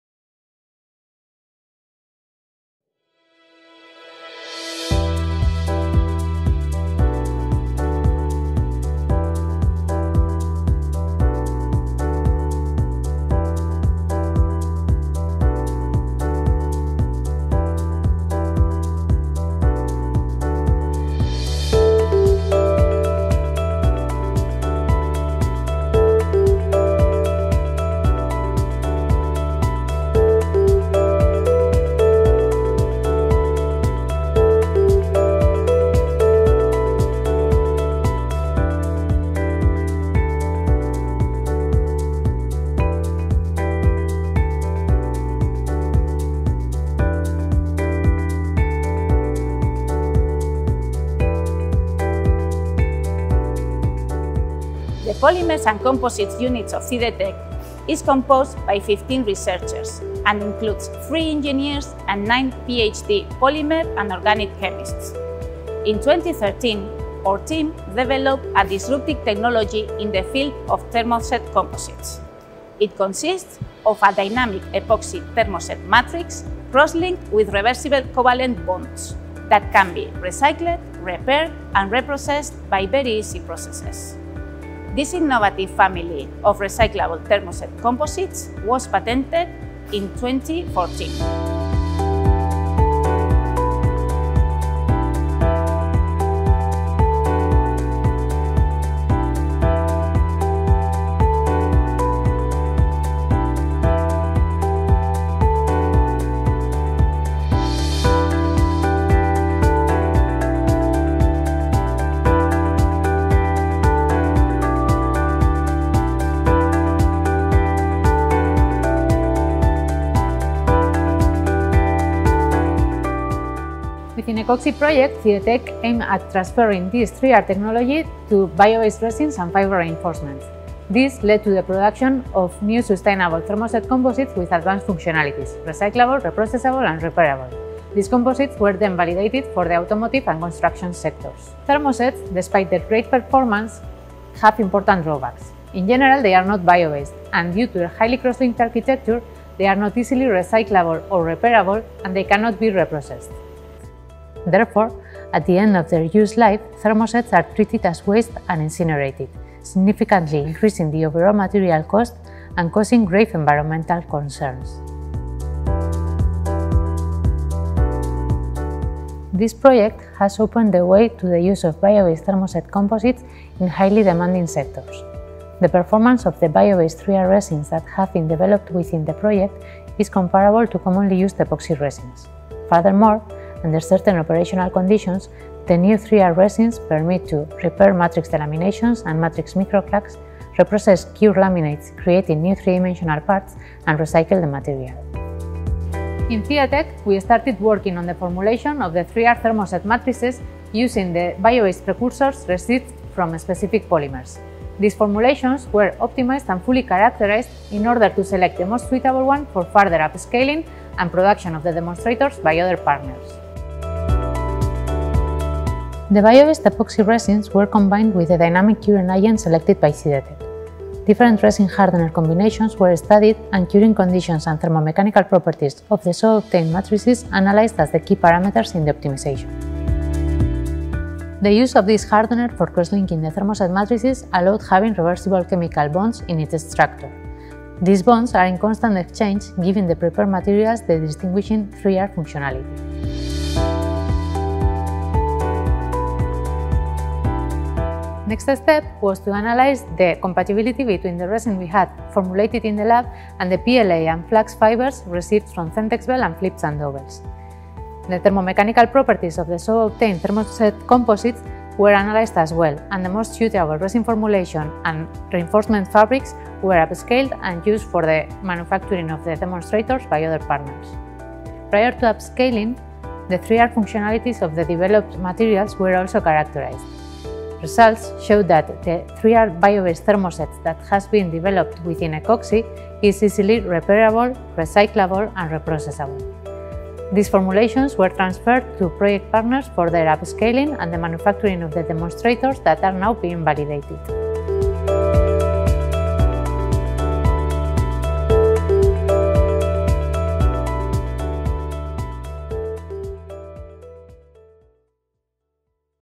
Episode 2: Developping novel bio-thermosets for a more sustainable future in composite materials.
During this period the main role of CIDETEC has been focused on the development of thermoset biocomposites to boost circular economy.

Within the ECOXY project, CIDETEC has expanded its patented 3R technology to bio-based epoxy resins and fibre reinforcements.

This has led to the production of new sustainable thermoset composites with advanced functionalities: repairable, reprocessable and reciclable, that can be used in sectors such as automotive or aerospace, amongst others.